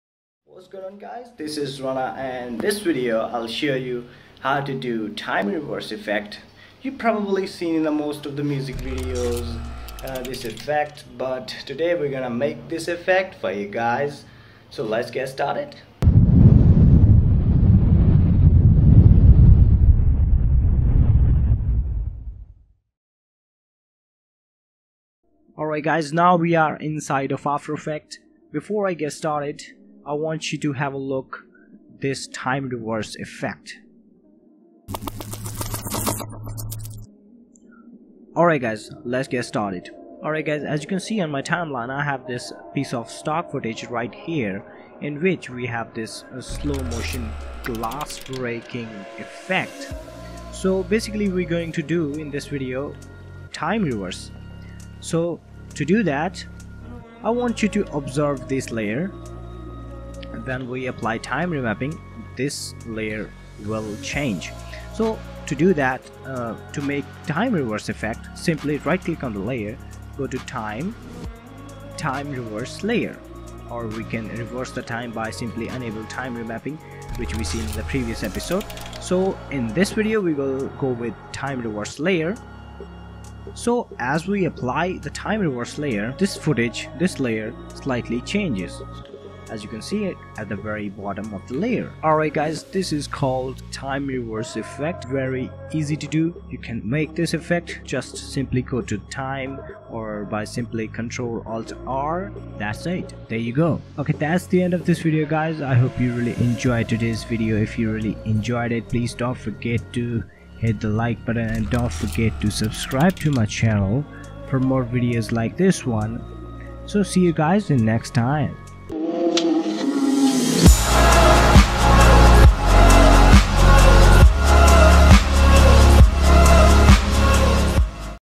After effects Tutorial || How to Reverse Time
In this video I'll show you how to Reverse Time in after effects

/c/RANAsVFX
•Sony Vegas Pro || Best Render settings at 30FPS & 60FPS for YouTube